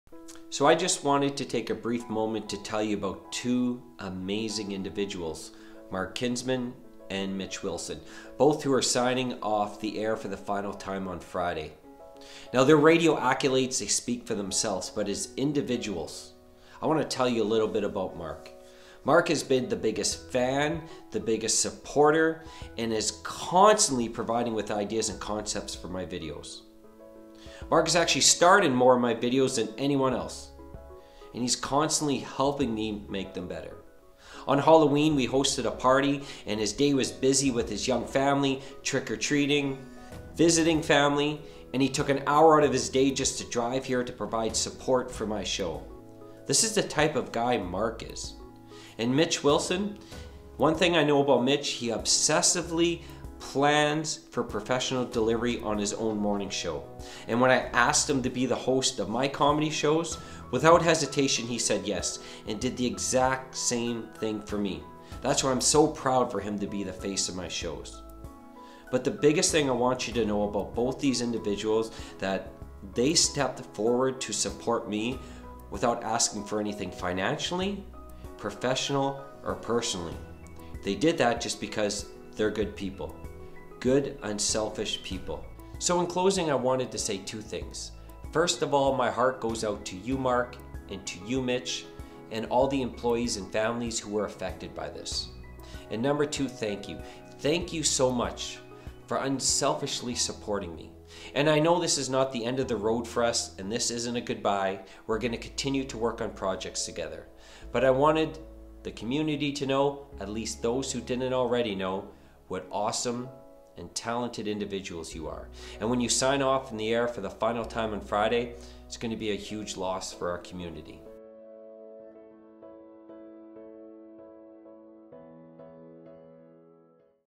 Dear Mark and Mitch
A thank you to the local radio hosts who have helped me along the way.